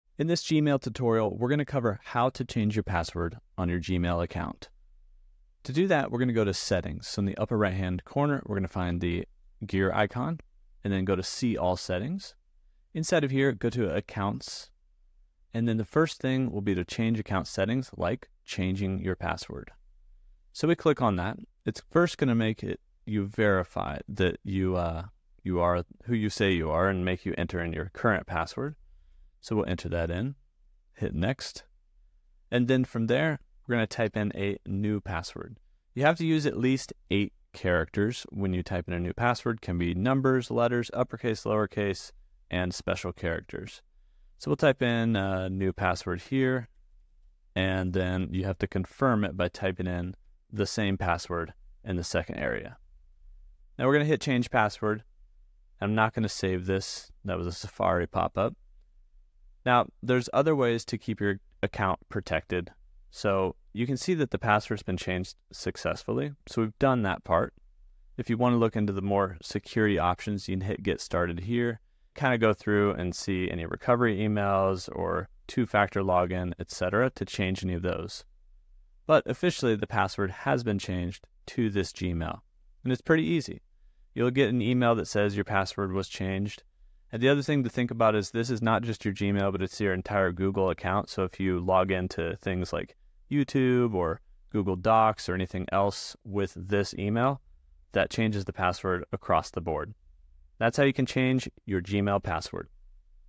How to Change Gmail Password
In this Gmail tutorial, learn how to change Gmail password. You can change your password in Gmail easily, but you'll need to know your current password.